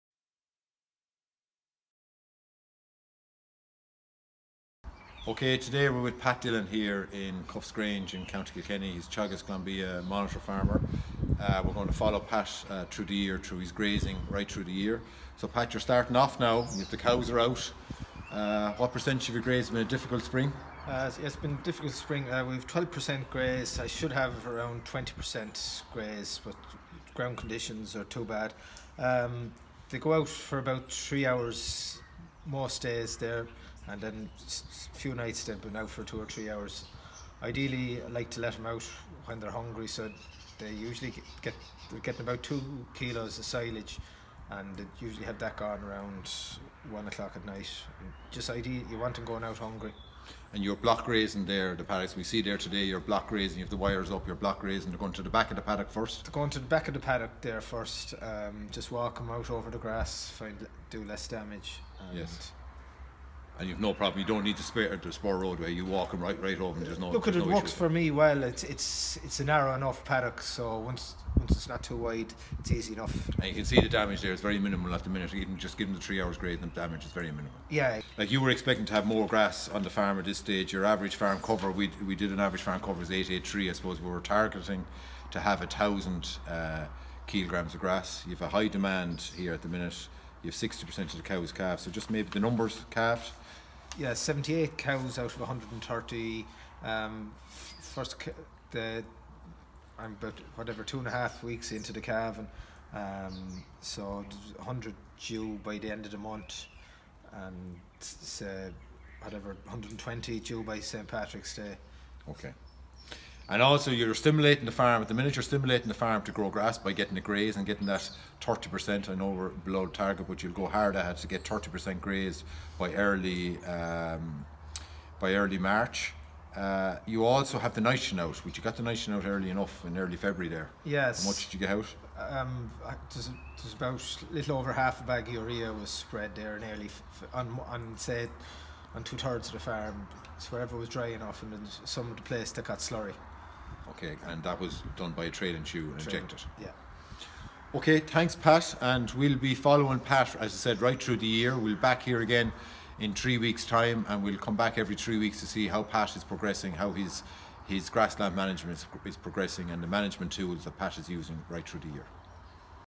Grass 1: Pat Dillon talks to Richard O'Brien about Grass Growth this February
Pat Dillon, Glanbia Ireland Monitor Farmer talks to Richard O'Brien, Teagasc about Grass Growth on his farm in Kilkenny.  We will be following Pats grass growth over the course of the season